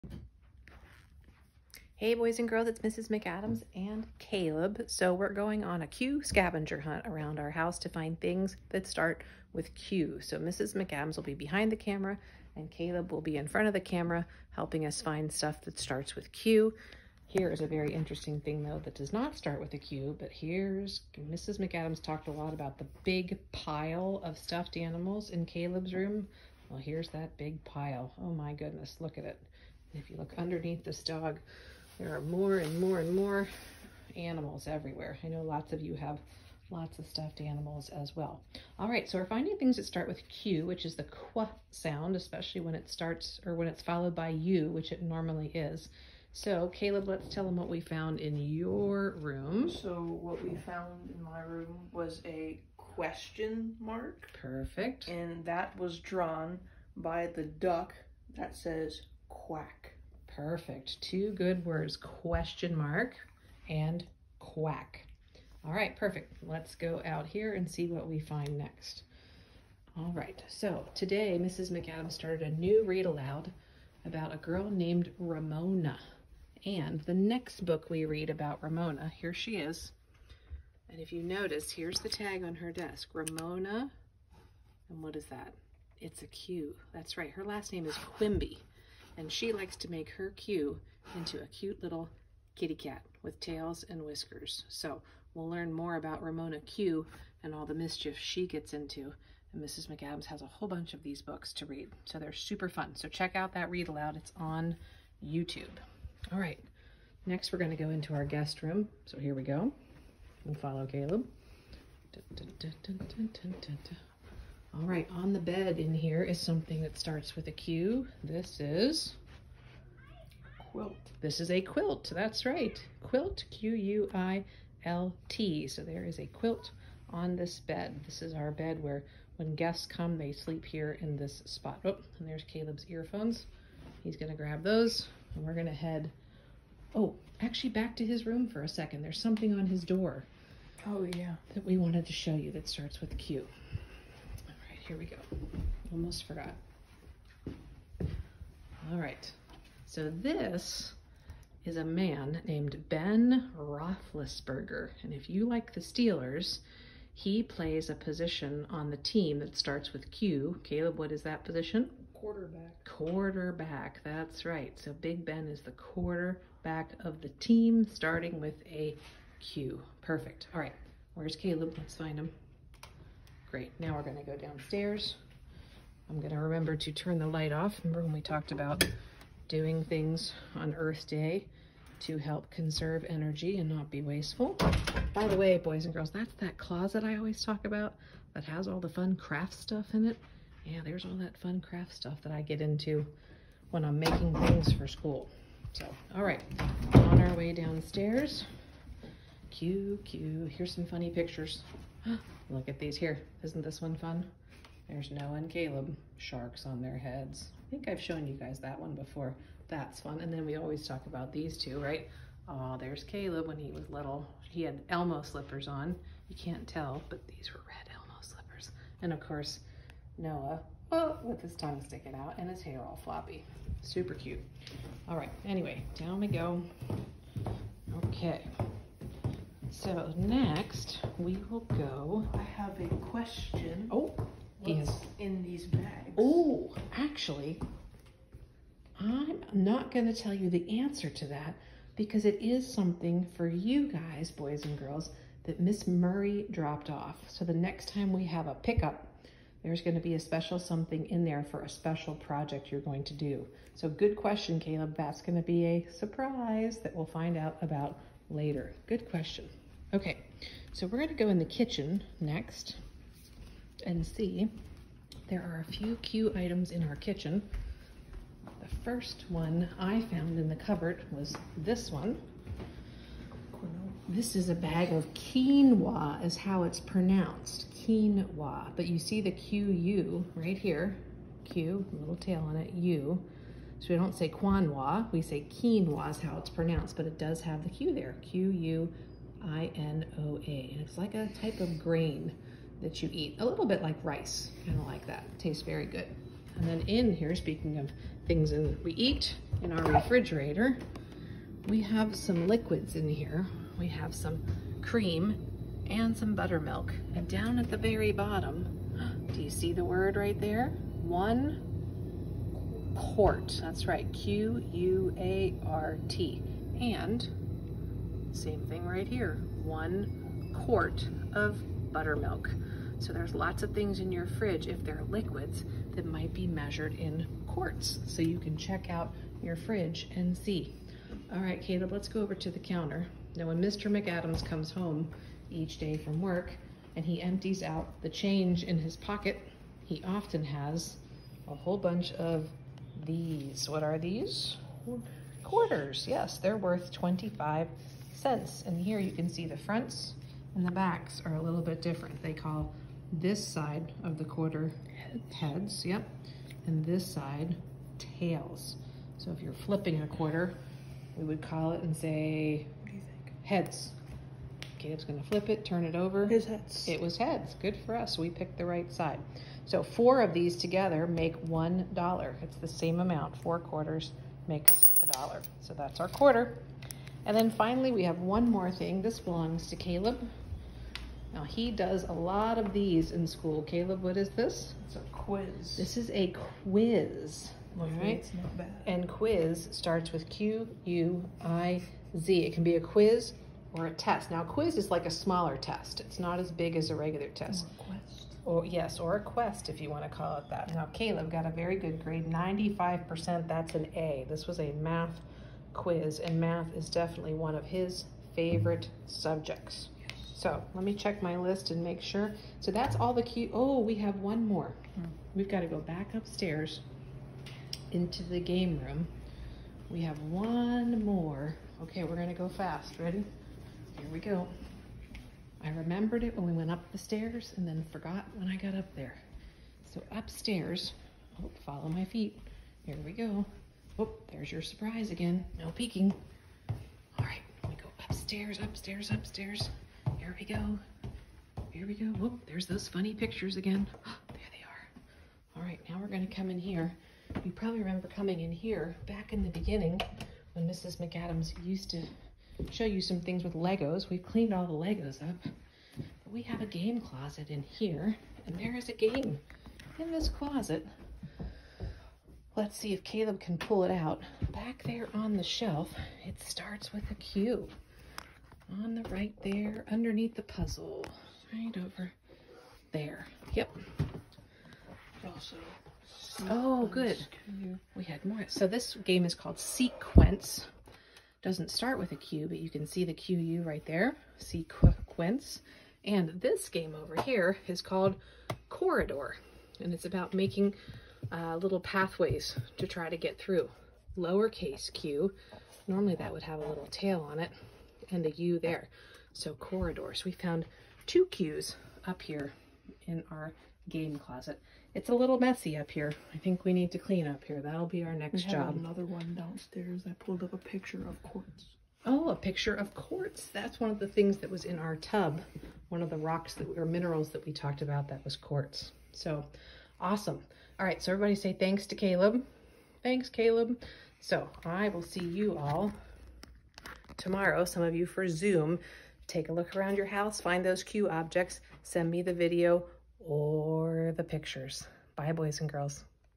Q Scavenger Hunt!
Come on a scavenger hunt for Q objects at Mrs. McAdams’ house!